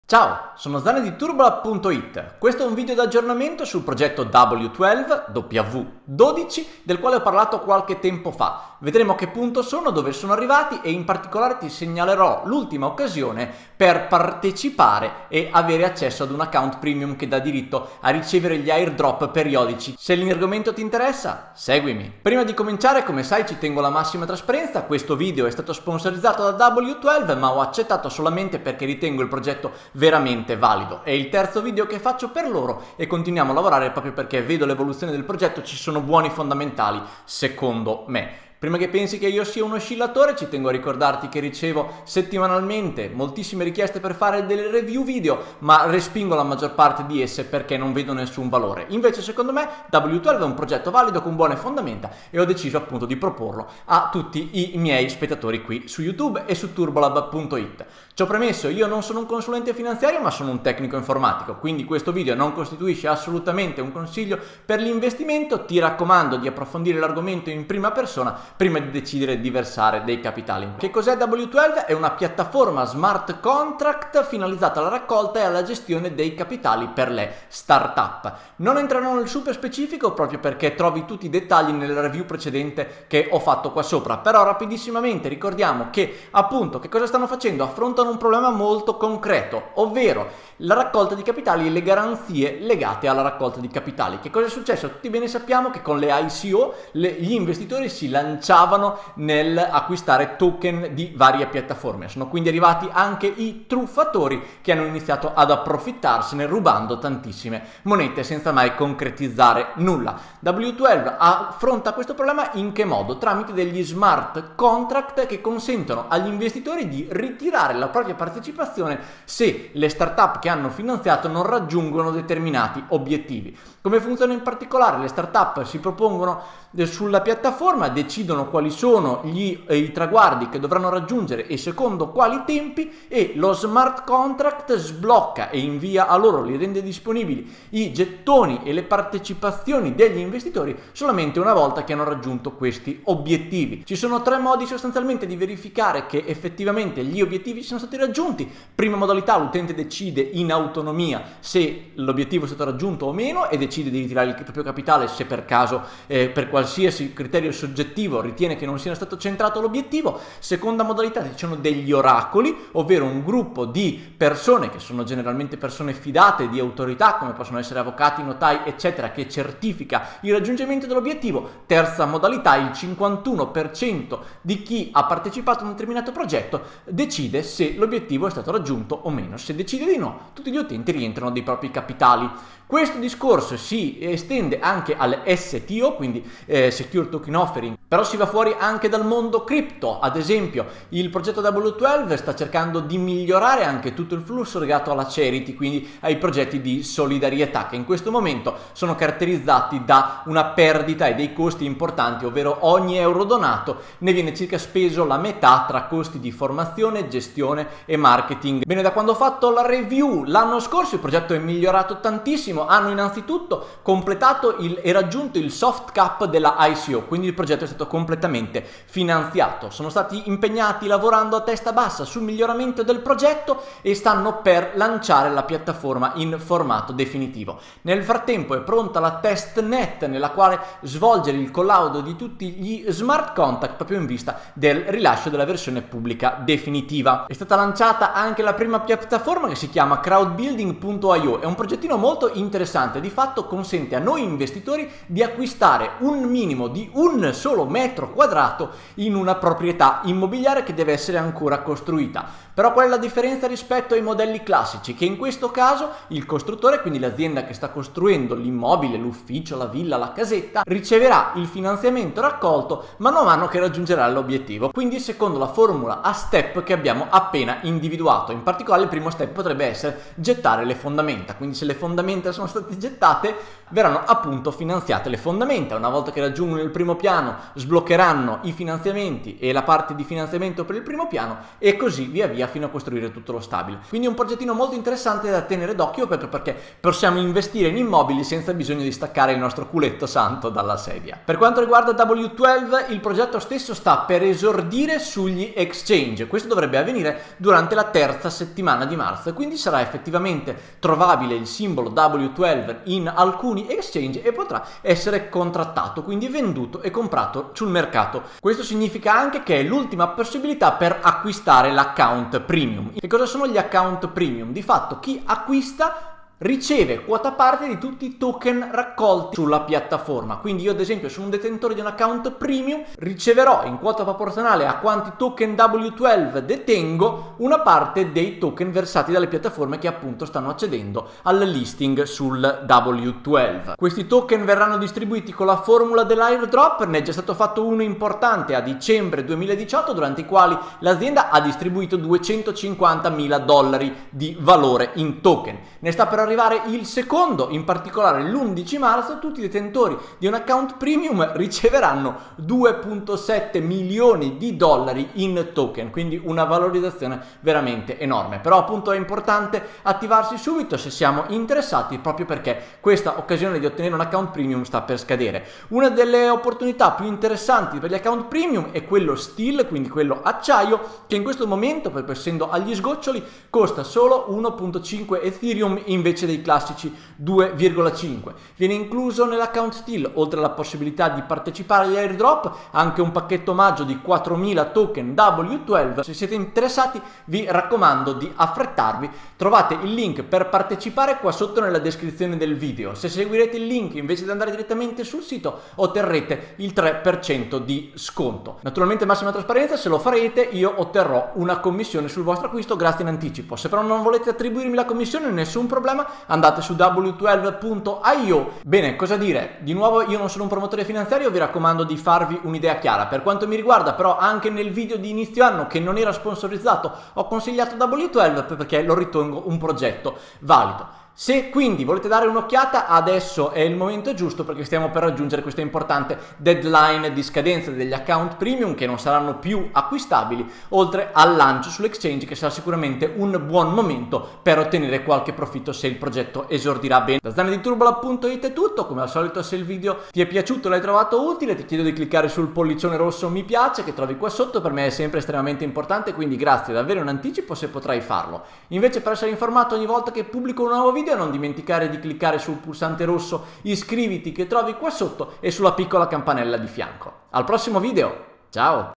💰 Cripto che mi piacciono: W12, la piattaforma Smart Contract di raccolta fondi
W12 è la piattaforma tecnologica che consente agli utenti di investire in ICO/STO e altri tipi di startup con la certezza di poter rientrare del proprio capitale in caso di truffa o mancato raggiungimento degli obbiettivi. Il token W12 arriverà sugli exchange già fra qualche giorno e, contestualmente, finirà definitivamente l'opportunità di ottenere un account premium tramite il quale partecipare agli airdrop periodici.

Nota: il video è stato ri-caricato per rispecchiare le nuove date prevista per il termine della vendita dell'account premium, l'airdrop e il debutto sugli exchange.